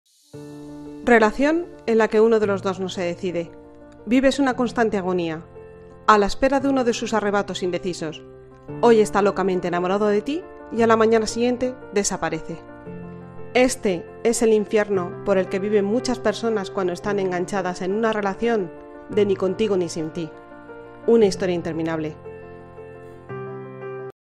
NI CONTIGO NI SIN TI una historia interminable
Relaciones a las que estas enganchada, ni contigo ni sin ti, una historia interminable.

Deja un like si te gusta el canal, me ayudaras a crecer, espero tus comentarios y que COMPARTAS este podcast con quien lo necesite.

VUELA alto mariposa.